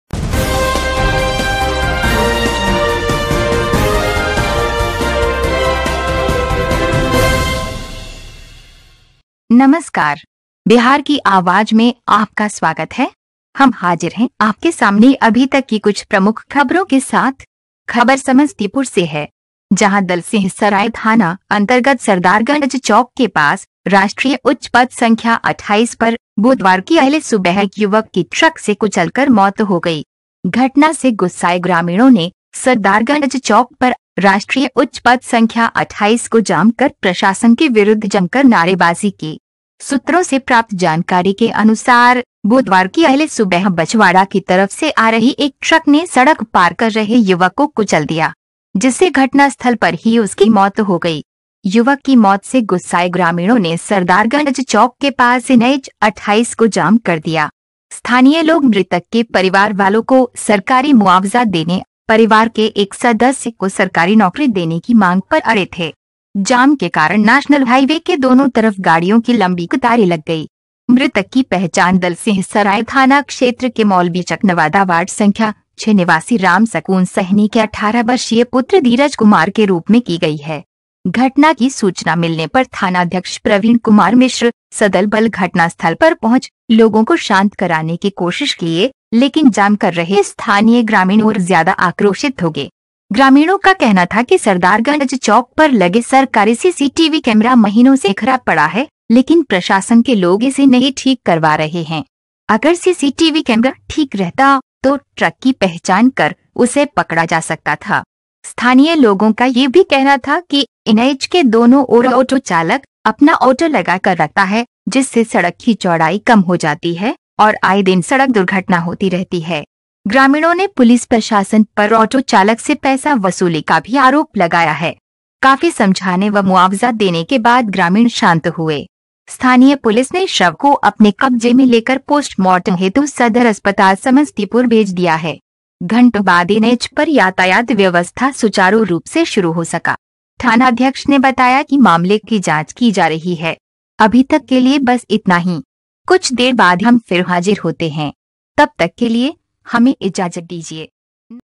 ट्रक से कुचलकर युवक की मौत, आक्रोशित लोगों ने किया एन एच 28 को जाम
दलसिंहसराय थाना अंतर्गत सरदारगंज चौक पर तेज रफ्तार ट्रक की चपेट मे आकर एक युवक की मौत हो गयी। इसके विरोध मे स्थानीय लोगों ने एन एच 28 को घंटो जाम कर दिया।